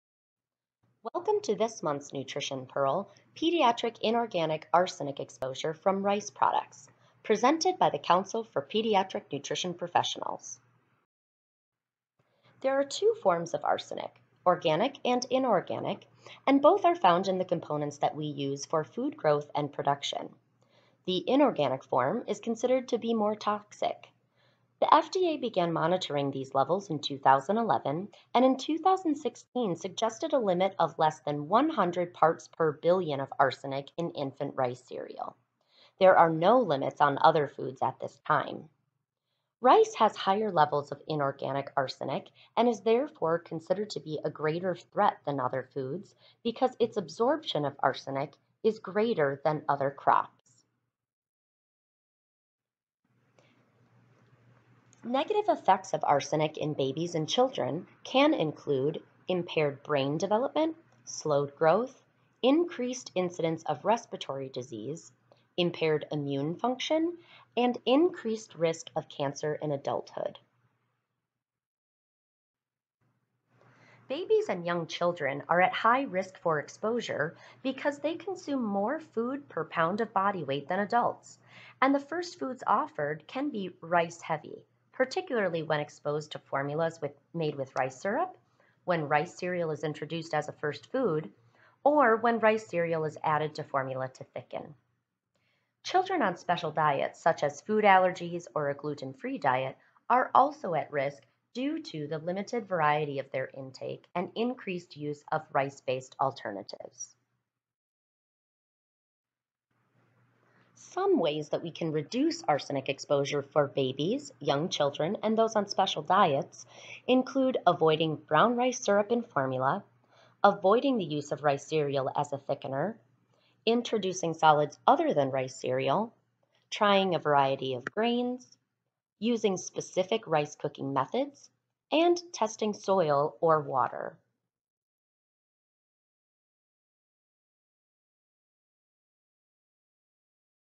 April 2019 Nutrition Pearl: Arsenic Exposure from Rice Products
This month, we tackle Arsenic Exposure from Rice Products, and offer alternative options